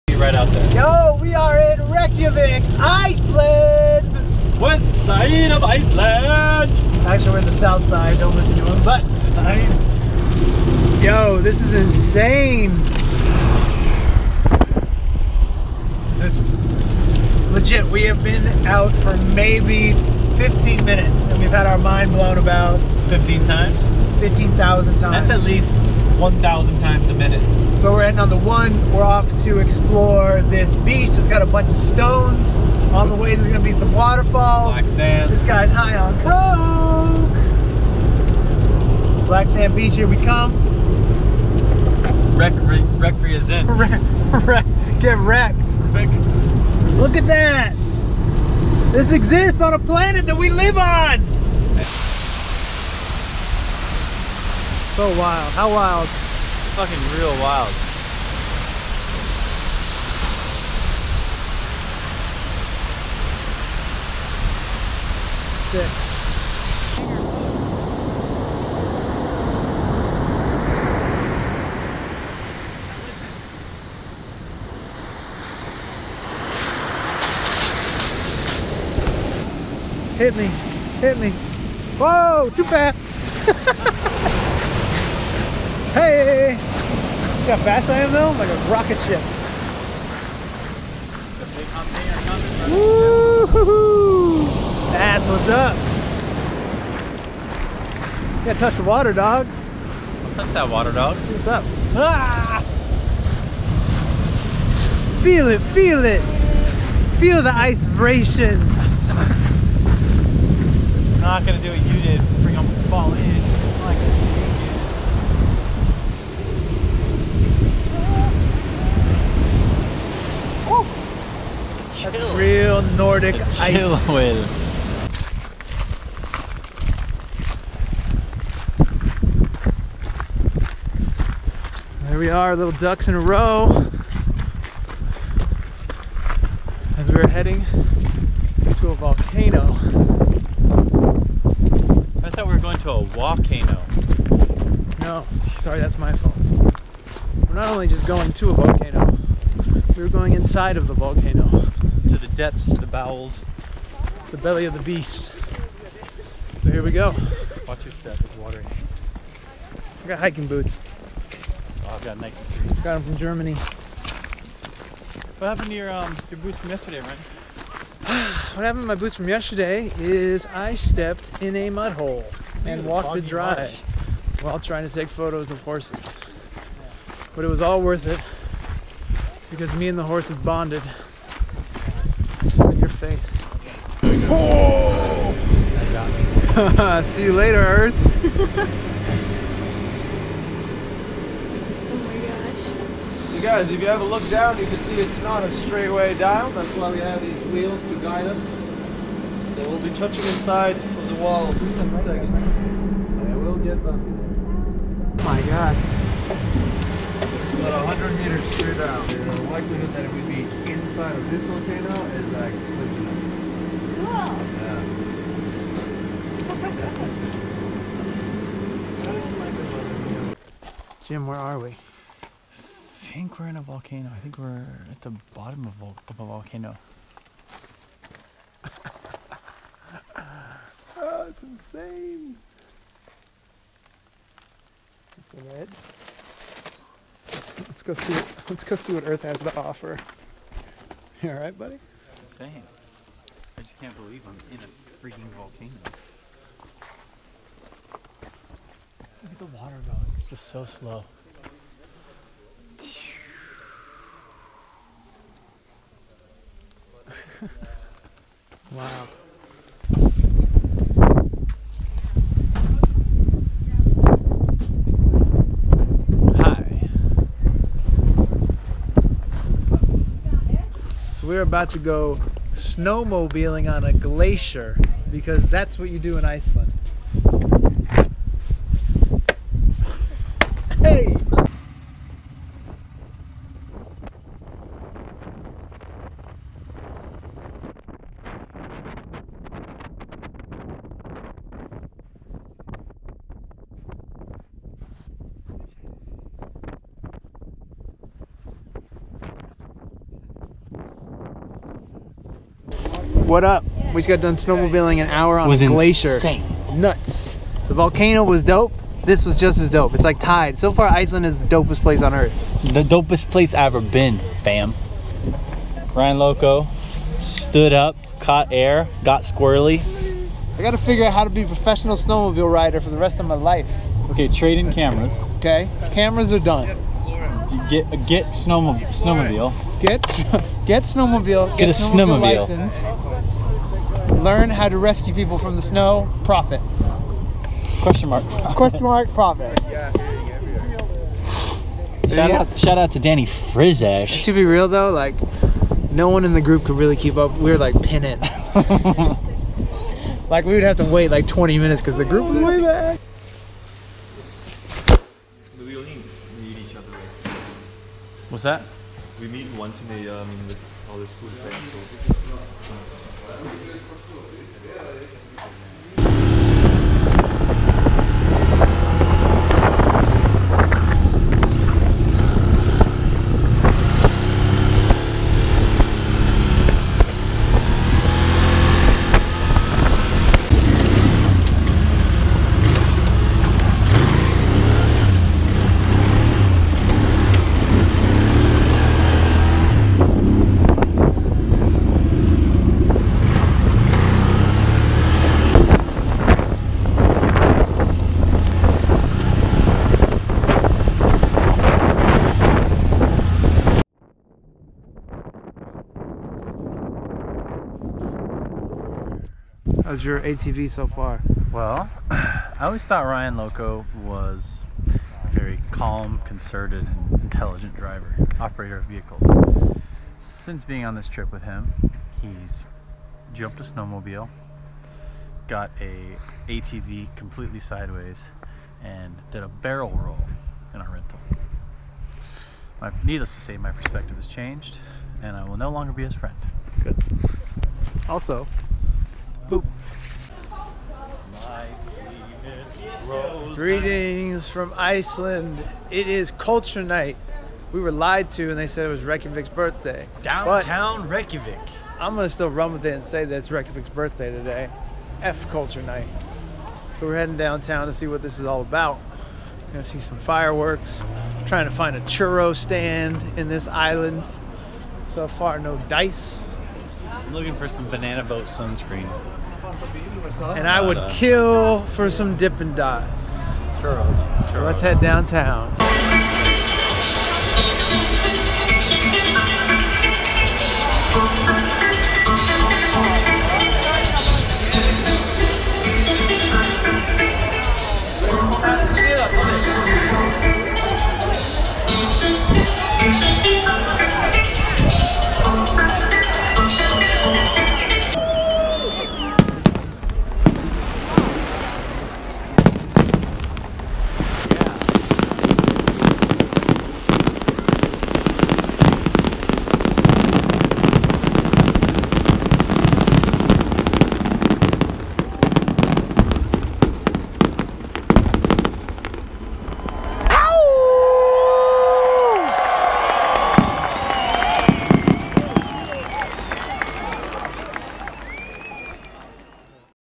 Iceland with James Law
Went to Iceland for a week with James Law. Waterfalls, volcanoes, snowmobiles and ATVs.